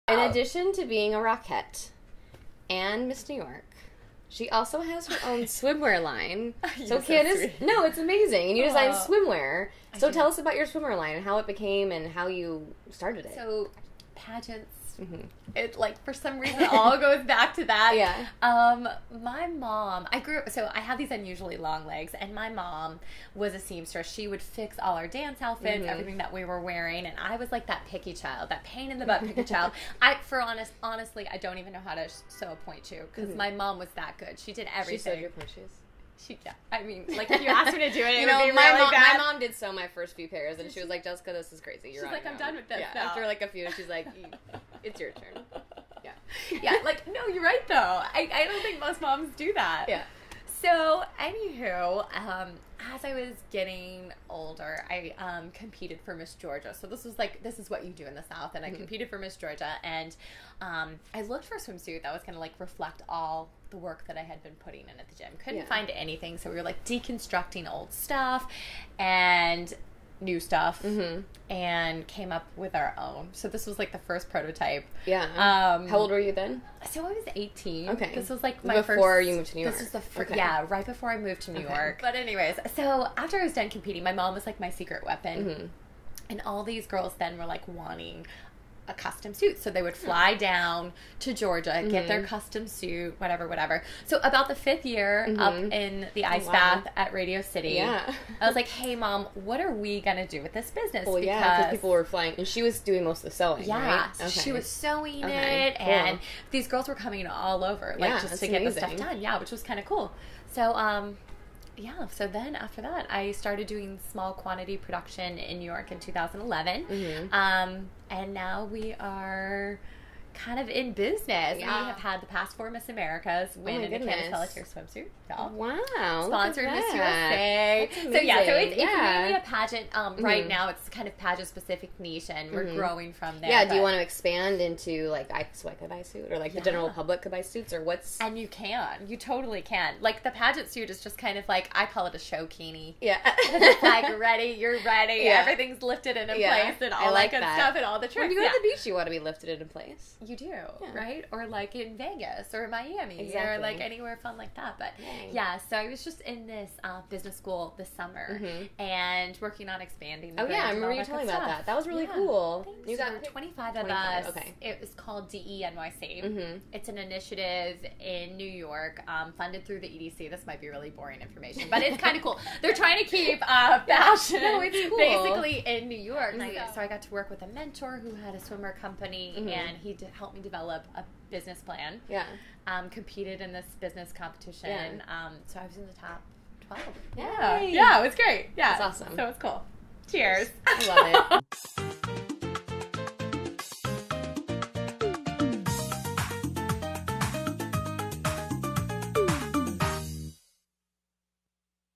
BARRE TALK - Episode 6 teaser "Kandice Pelletier Swimwear"
In this teaser from Episode 6 Kandice tells me how she started her swimwear line Kandice Pelletier Swimwear!

The past 5 Miss America's have worn her suits and so can you!